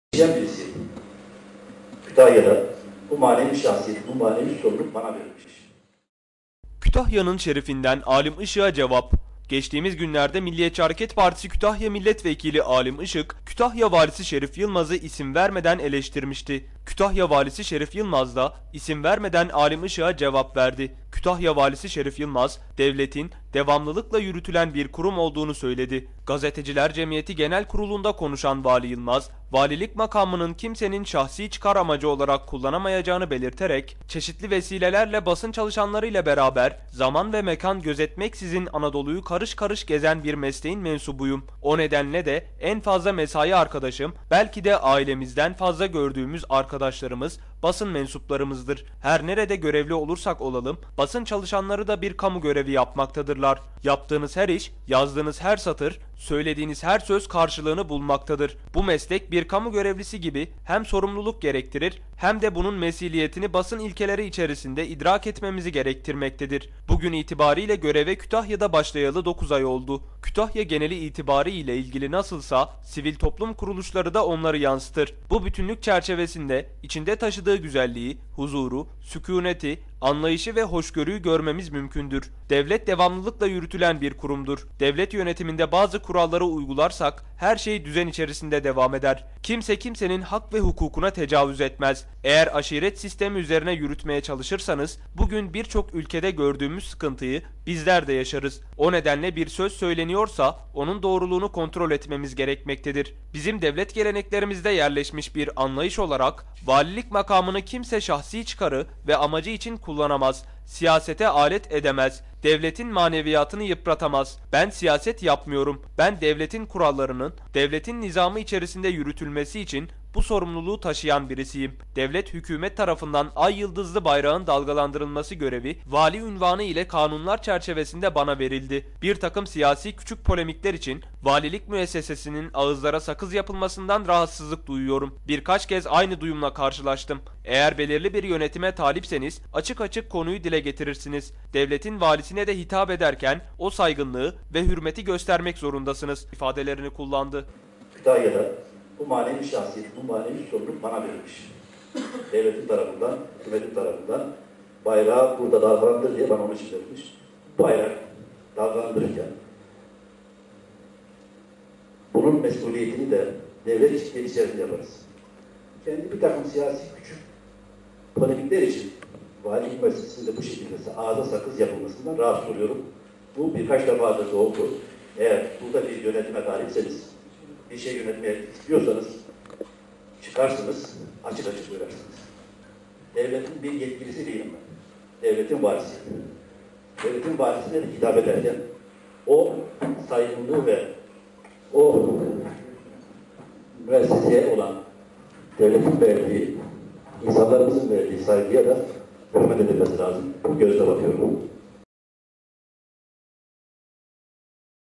" VALİLİK MAKAMINI KİMSE ŞAHSİ ÇIKARI VE AMACI İÇİN KULLANAMAZ "
VALİ ŞERİF YILMAZ' DAN ALİM IŞIK' A CEVAP